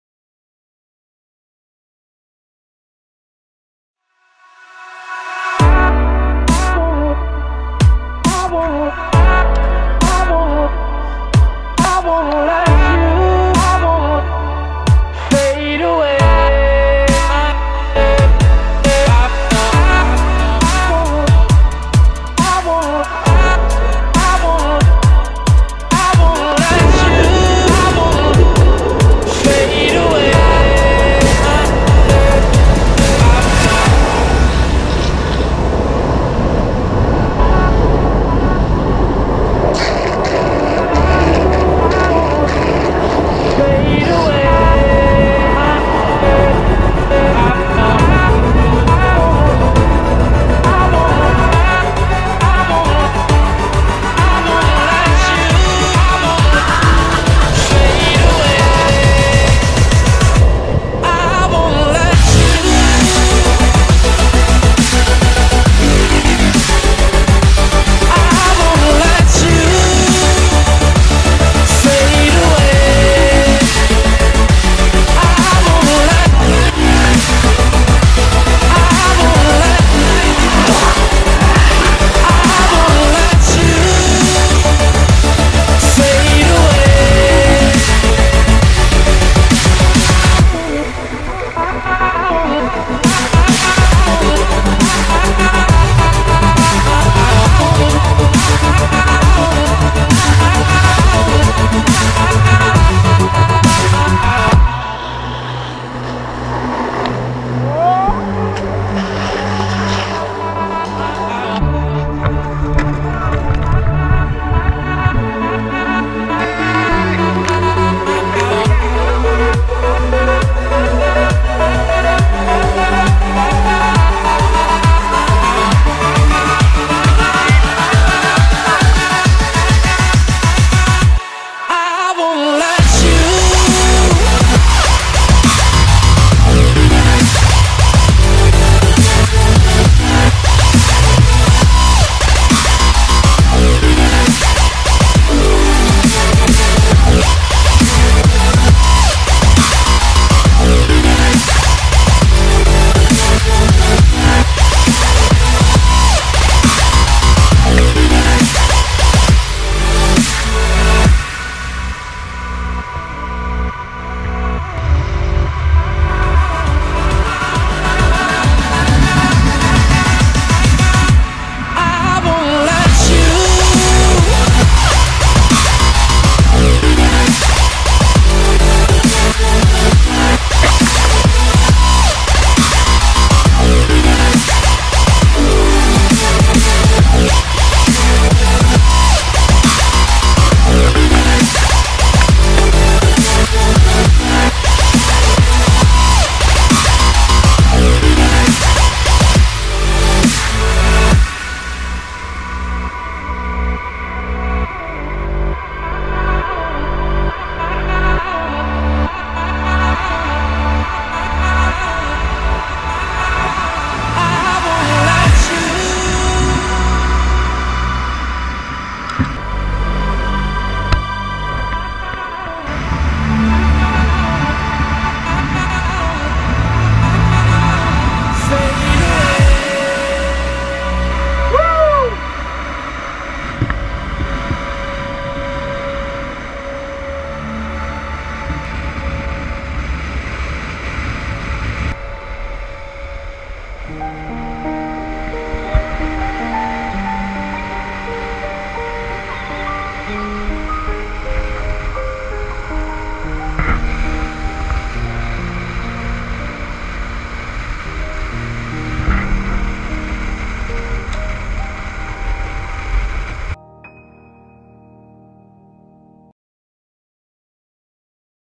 Roban Longboards # Wallonhill 2015 downhill skateboarding
Wallonhill festival downhill skateboarding 2015
filmed with: Gopro hero 3 White
location: Houyet, Belgium